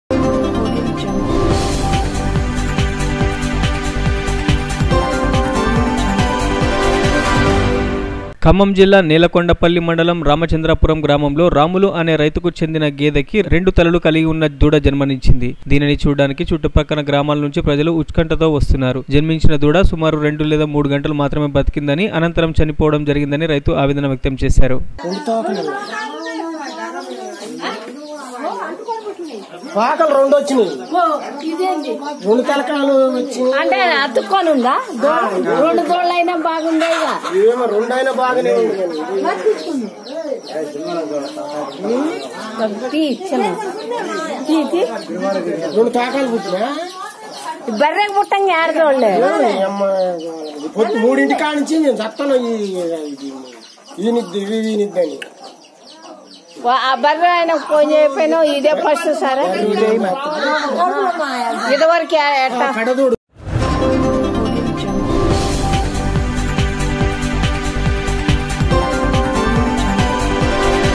ఖమ్మం జిల్లా;- రాములు అనే రైతు కు చెందిన గేదె కి రెండు తలల దూడ జన్మించింది.
ఖమ్మం జిల్లా నేలకొండపల్లి మండలం రామచంద్రాపురం గ్రామంలో రాములు అనీ రైతు కు చెందిన గేదె  కి రెండు తలలు కలిగివున్న దూడ జన్మించింది.